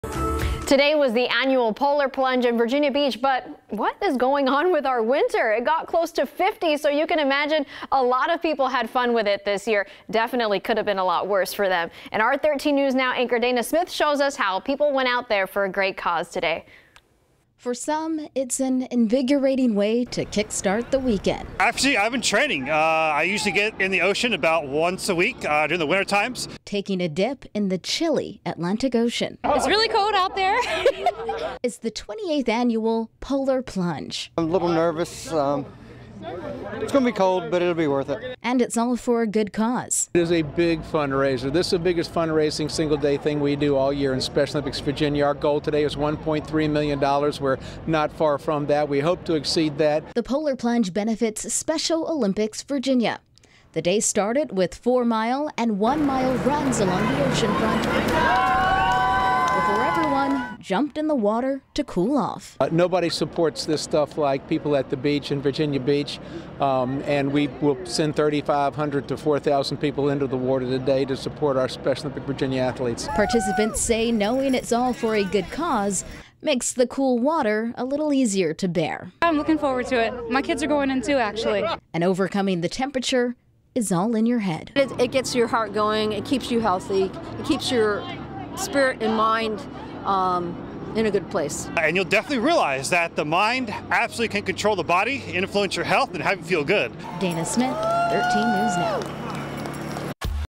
Scores of people take polar plunge at Oceanfront to benefit Special Olympics
The Saturday event starts with a 4-mile run and ends with a chilly dip into waters at the Oceanfront.